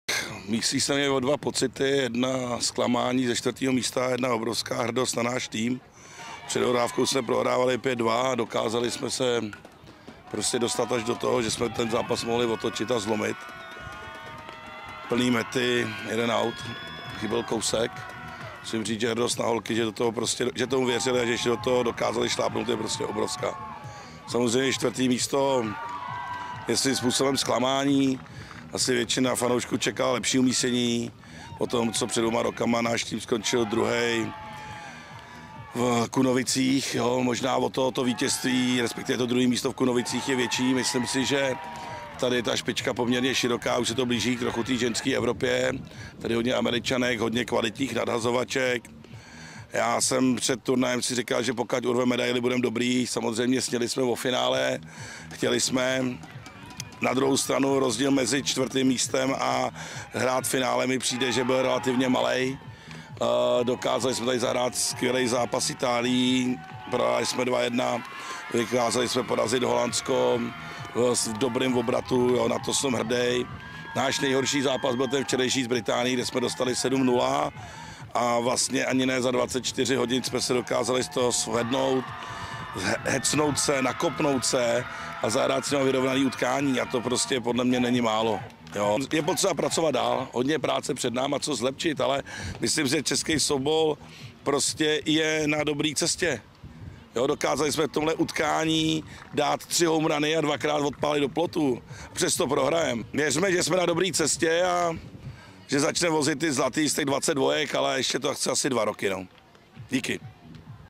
ME U22 2024 | Závěrečné hodnocení s Tomášem Kusým
Hlavní trenér reprezentace U22 hodnotí 4. místo na mistrovství Evropy, kvalitu soupeřů a dnešní boj o bronzovou medaili. Jaké byly pocity po zápase s Velkou Británií? A jak velký rozdíl je mezi týmem na čtvrtém místě a finalisty šampionátu?